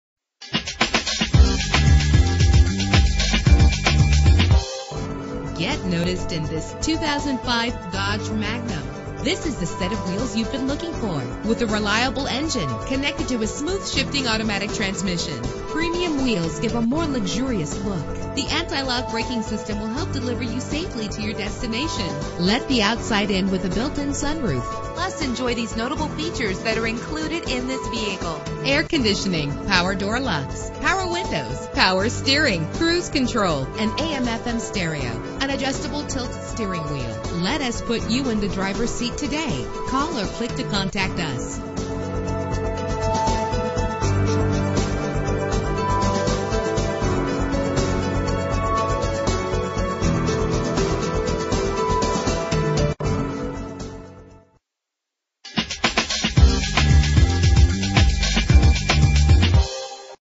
2005 Dodge Magnum - Whitefish MT
Year: 2005
Make: Dodge
Model: Magnum
Trim: SXT Sport Wagon 4D
Engine: 6 cylinder HO 3.5 Liter
Transmission:
Color: Silver
Mileage: 60738
Address:
6331 HWY 93 South